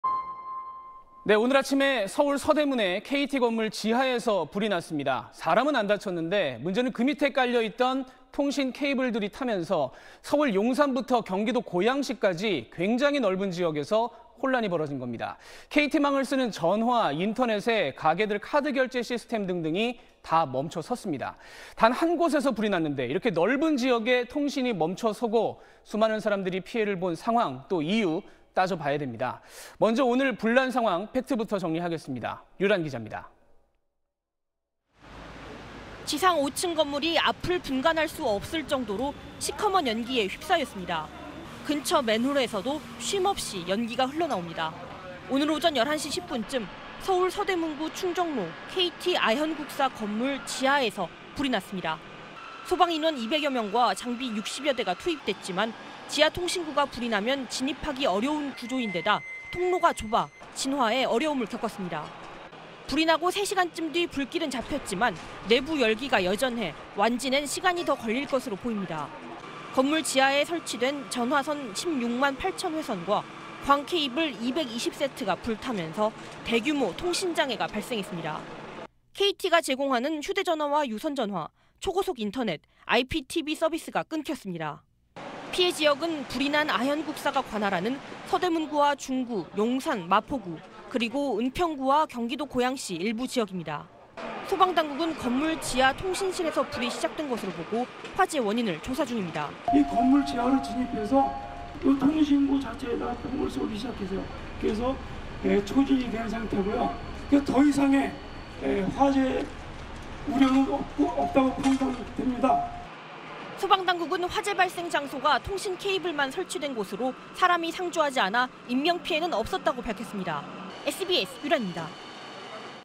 서울 5개 구 통신대란 부른 'KT 화재'…원인 조사 착수 / SBS
오늘(24일) 아침에 서울 서대문에 KT 건물 지하에서 불이 났습니다. 사람은 안 다쳤는데 문제는 그 밑에 깔려있던 통신 케이블들이 타면서 서울 용산부터 경기도 고양시까지 굉장히 넓은 지역에서 혼란이 벌어진 겁니다. KT 망을 쓰는 전화, 인터넷에, 가게들 카드 결제시스템 등등이 다 멈춰 섰습니다. 단 한 곳에서 불이 났는데 이렇게 넓은 지역의 통신이 멈춰 서고 수많은 사람들이 피해를 본 상황, 이유 따져봐야 합니다. 먼저 오늘 불난 상황 팩트부터 정리하겠습니다.
류란 기자입니다.